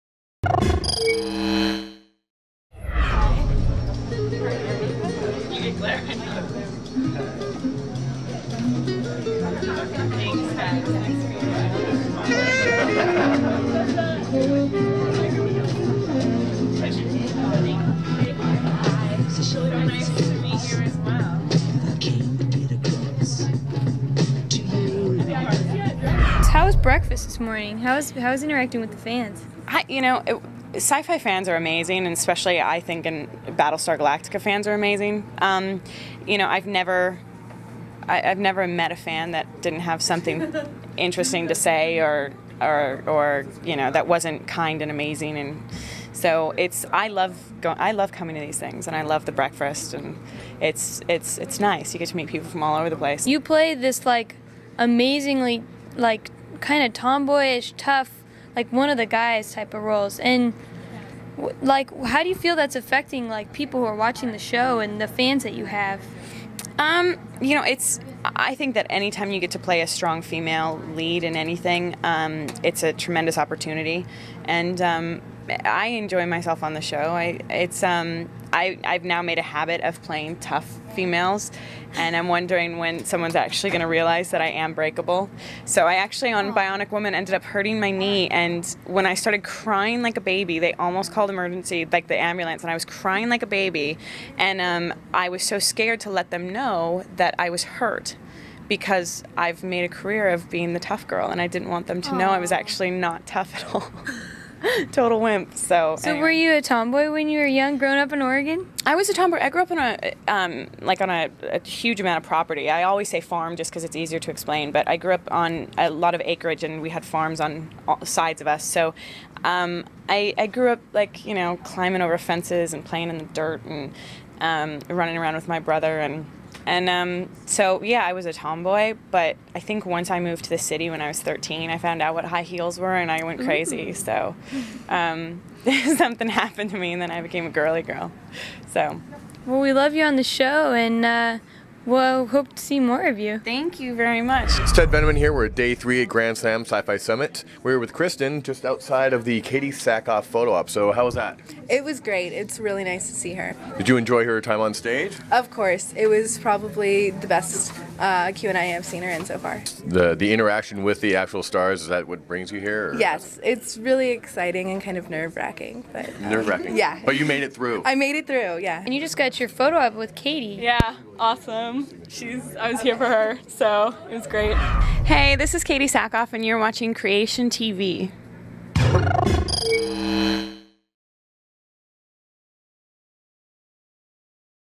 Katee Sackhoff at the Grand Slam Sci-Fi Event-07
for information about upcoming conventions. Katee Sackhoff takes photo ops with fans at The Grand Slam Sci-fi Summit Evevt-2007. Interview with Creation TV